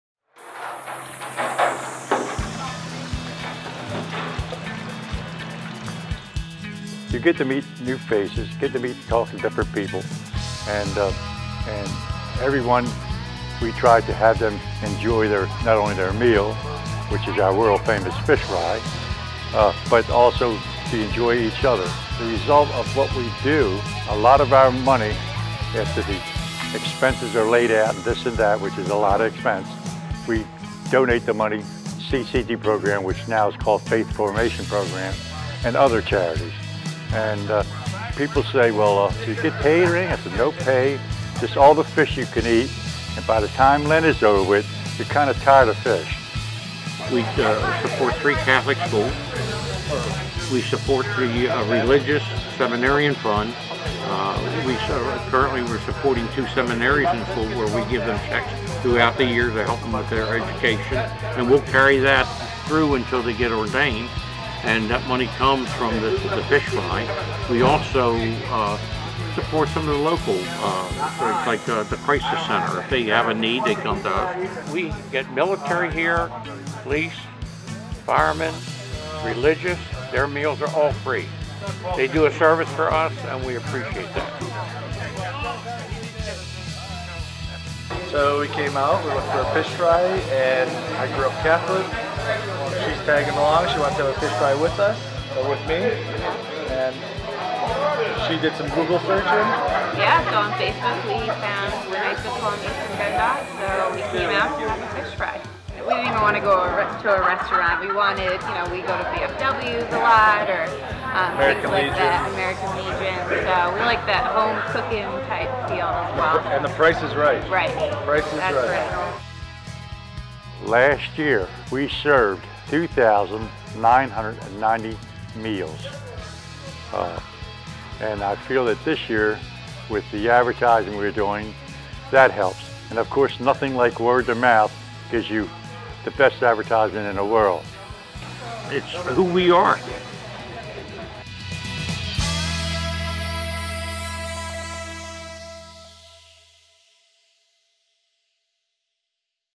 World-Famous Fish Fry in Dundalk
The Knights of Columbus sponsor a 'World-Famous' Fish Fry in Dundalk on the Fridays of Lent.  For more information about their event and other fish frys in the Archdiocese of Baltimore, go